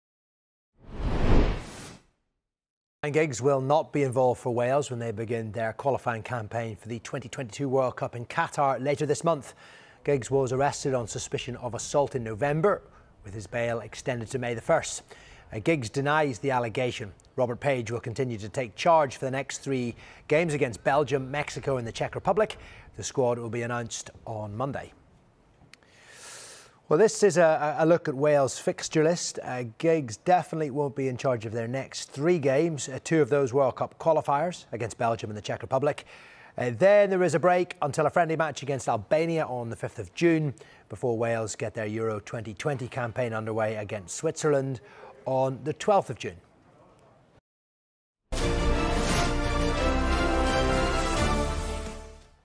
Ryan Giggs will not be involved in Wales' upcoming international camp
Ryan Giggs will not be involved in Wales' international camp when they start their qualifying bid for the 2022 World Cup later this month. Giggs was previously arrested for suspicion of assault, he denies the allegations.